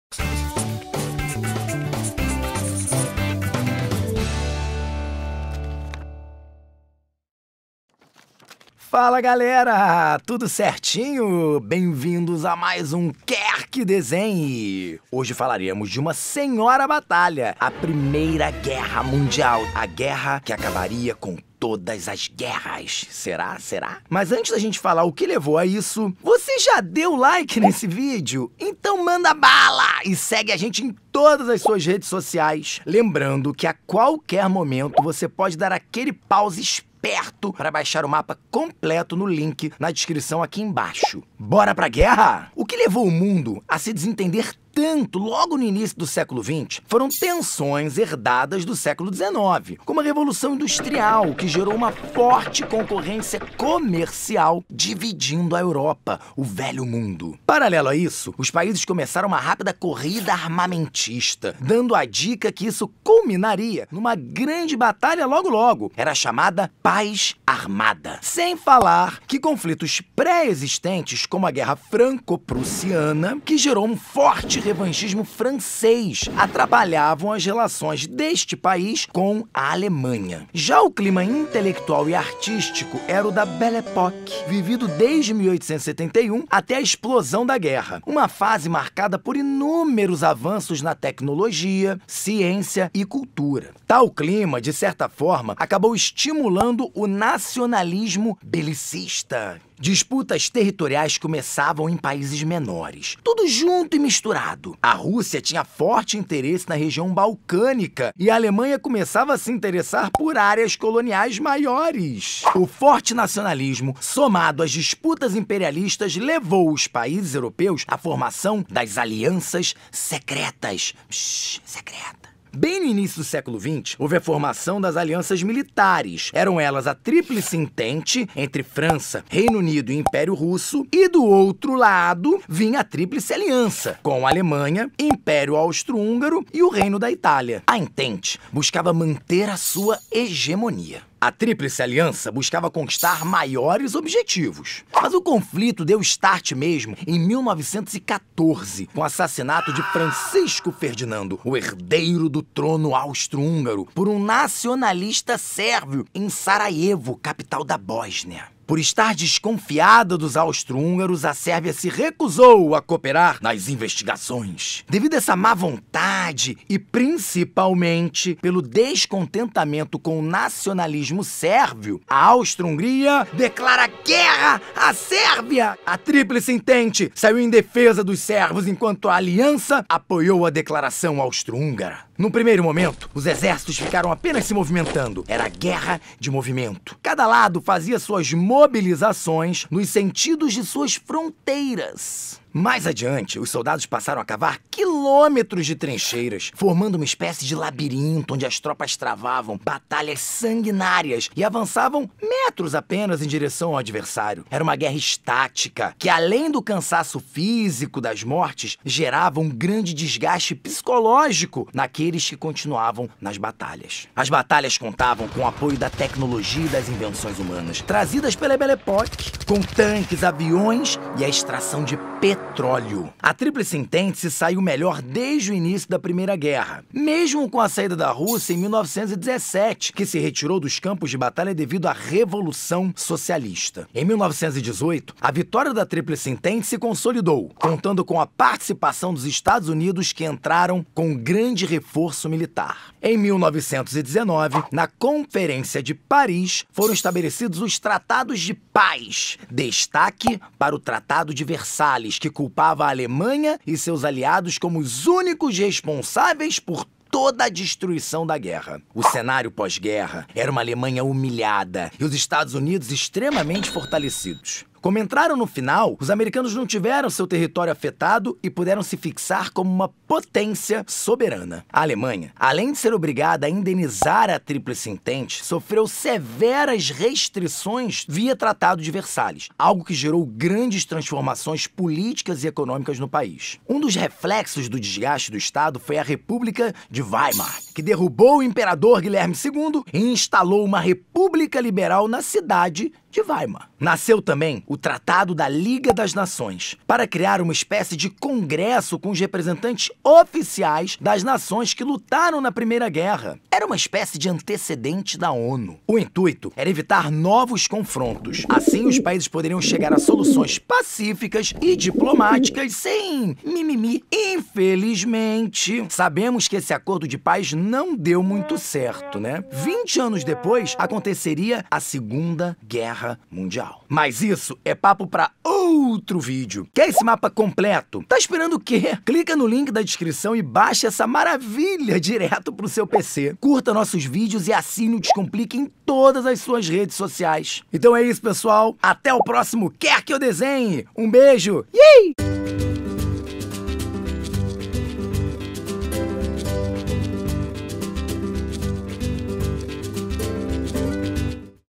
PRIMEIRA GUERRA MUNDIAL: RESUMO | HISTÓRIA | QUER QUE DESENHE?
Neste vídeo do Quer Que Eu Desenhe nós vamos te contar sobre o contexto e as consequências da Primeira Guerra Mundial.

A Primeira Guerra Mundial abalou nações e deixou marcas profundas na humanidade. Ao compreender a complexidade do conflito, somos convidados a refletir sobre as consequências devastadoras e os sacrifícios humanos enfrentados nesse período.

No Enem, esse tema sensível e crucial pode ser abordado, destacando a importância de compreender os eventos e contextos que levaram ao conflito. Vamos embarcar nessa jornada histórica e compreender as implicações da Primeira Guerra Mundial. Resumo maneiro é só aqui.